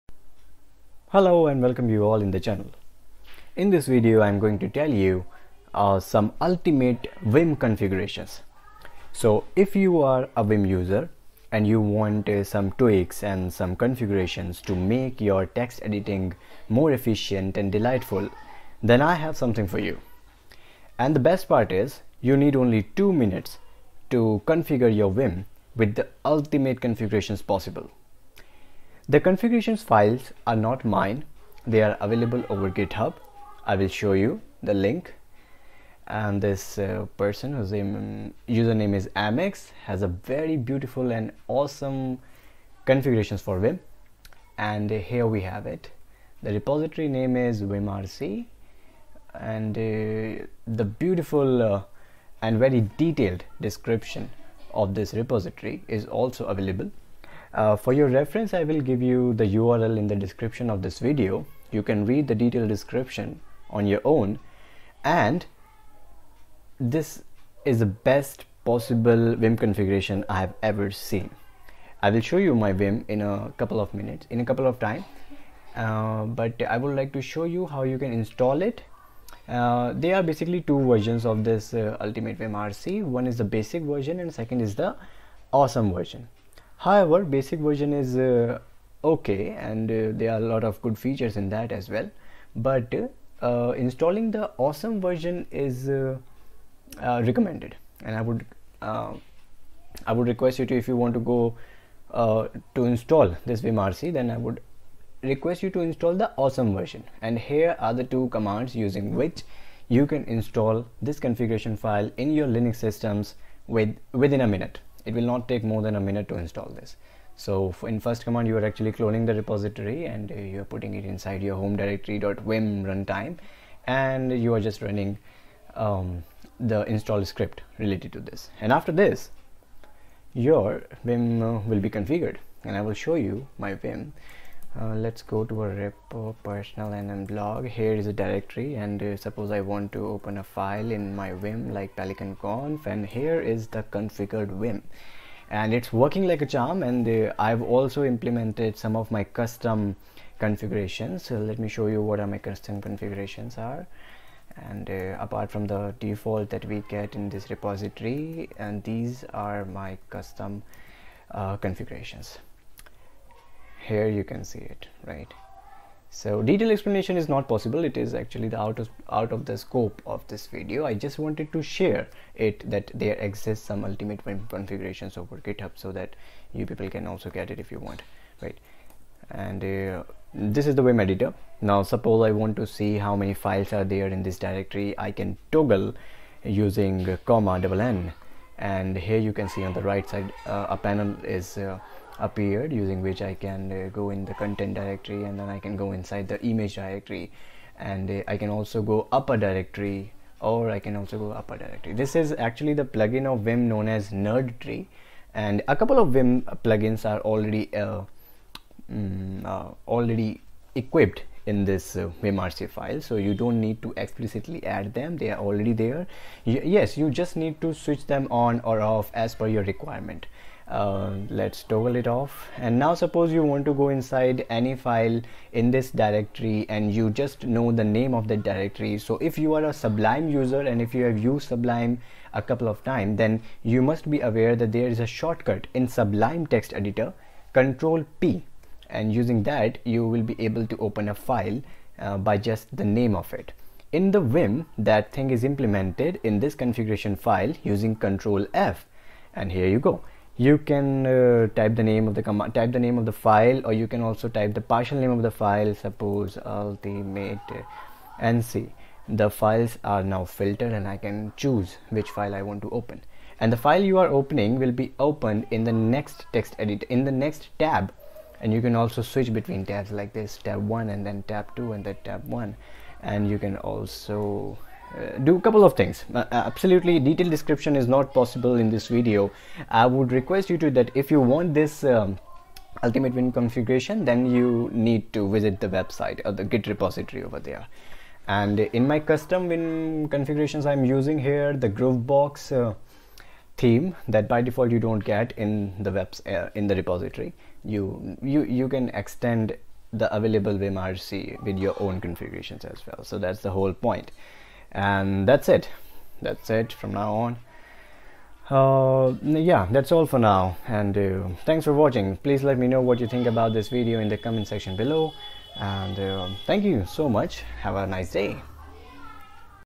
The Ultimate Vim Configuration
Hey!

In this video, find out how to enable the ultimate configurations for your Vim text editor. URL of the repository "The Ultimate Vim Configuration" is

Specifications:
System: Ubuntu20.04 LTS (focal-fossa amd64)
Vim: 8.1.0